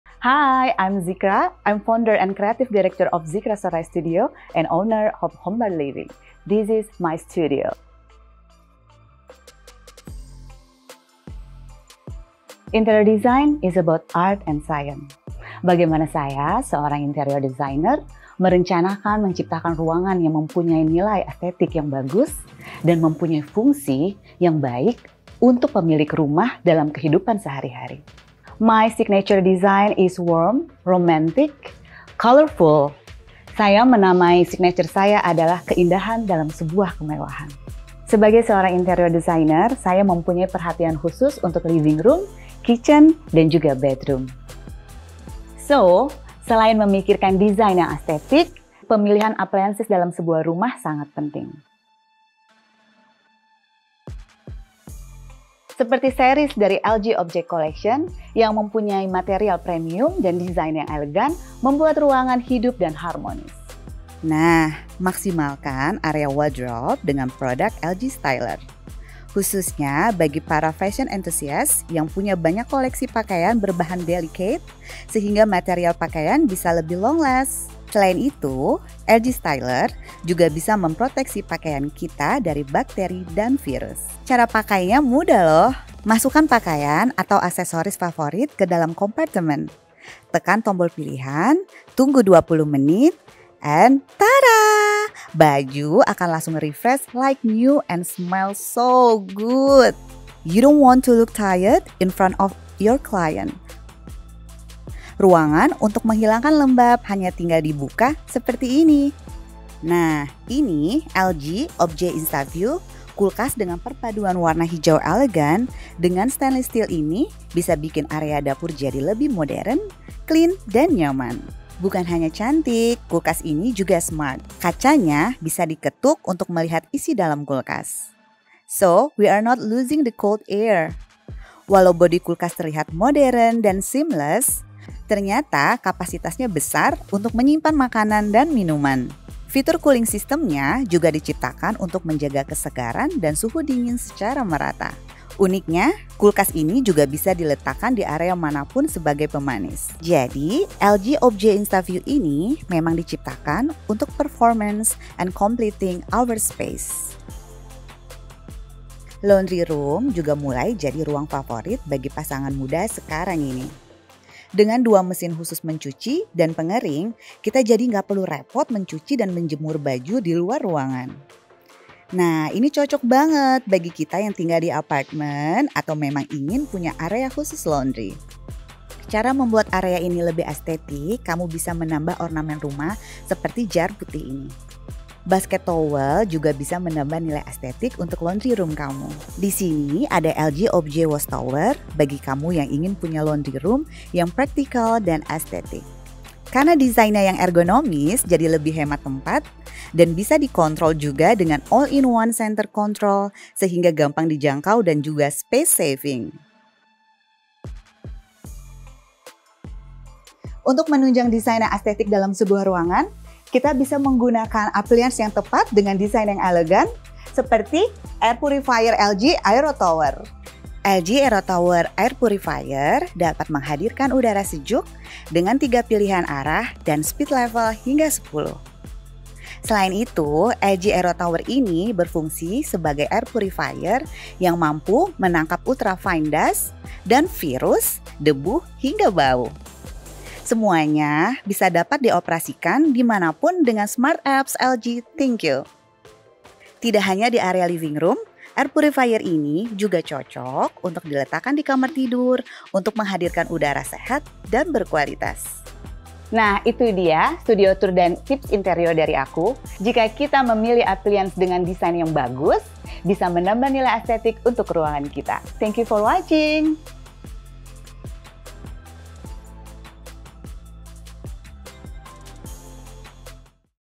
Studio Tour Zikra Soraya, Founder Home Bar Living
Interior desainer, Zikra Soraya, mengajak Her World Indonesia, untuk berkunjung ke studionya, Home Bar Living, berlokasi di area Kemang. Zikra juga membagikan tips interior menggunakan pemilihan home appliances yang tepat dari LG Indonesia untuk menciptakan ruangan yang mewah dan stylish, namun tetap fungsional.

Content writer: Fani Tanggara & Kiki Priskila
Videographer: Iman Syayogya & Putri Hanifa
Video editor: Afif
Penata Gaya: Girah Ababyl
Makeup: Linda Kusumadewi
Hairdo: Winda
Wardrobe: Uniqlo